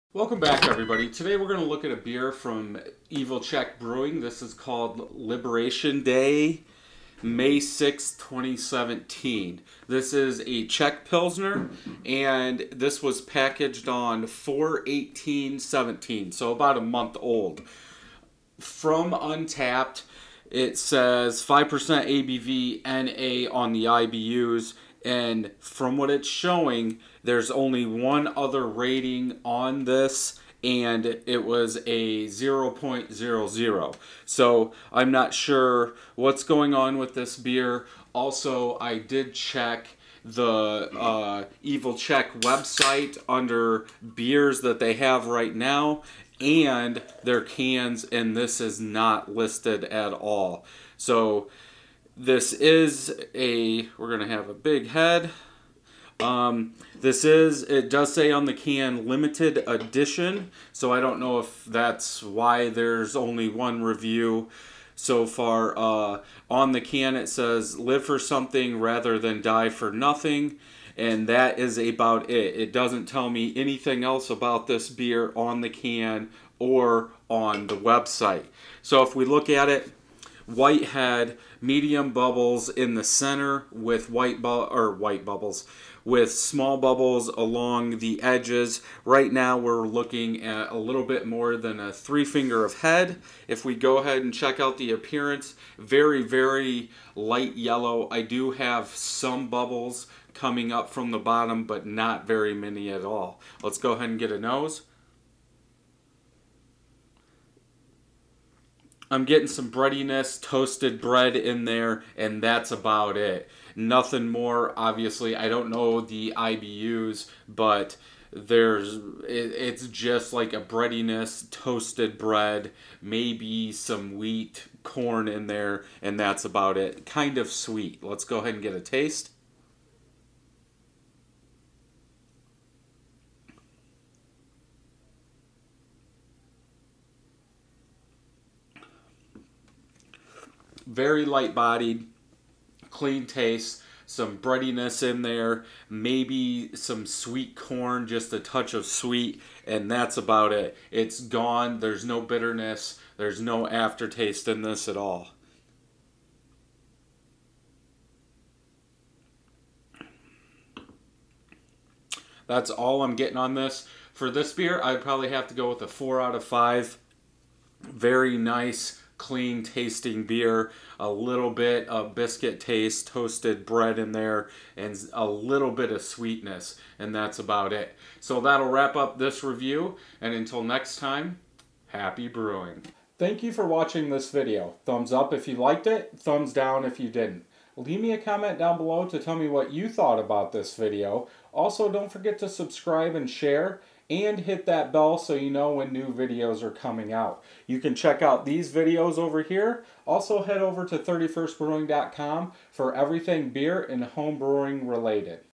Evil Czech Liberation Day Beer Review
Today's beer review is Liberation Day May 6 from Evil Czech Brewery. This is a Czech Pilsner. On info for the beer on the website.

5% ABV
N/A IBUs

What to help keep the beers flowing with out spending money? Check out Panel App.

" Bells Brewery Oberon Ale 2018 American Wheat Ale Beer Review "